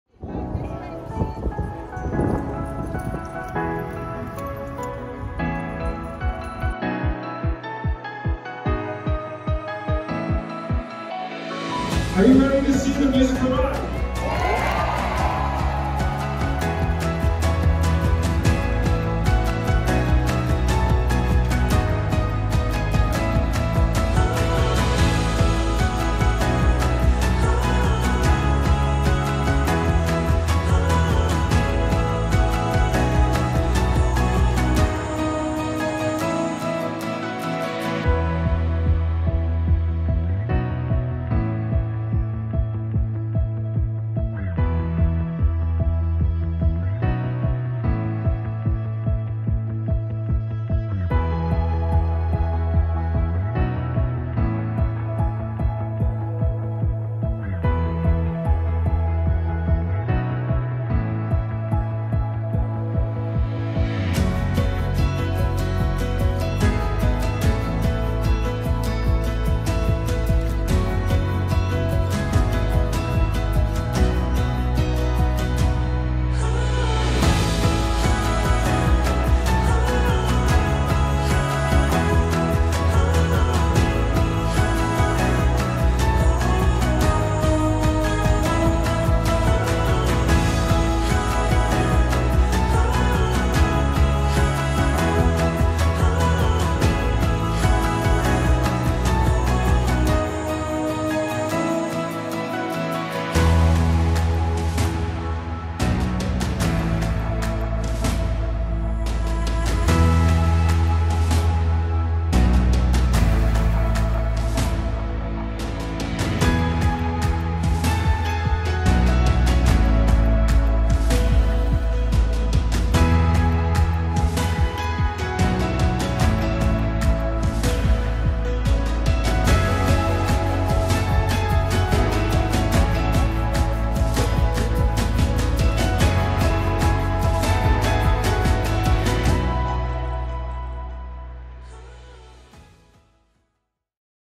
The RCMP Musical Ride in Burnaby, BC on June 26, 2023
The world-famous RCMP Musical Ride thrilled spectators during a sold-out show on June 26, 2023 at Burnaby's Swangard Stadium.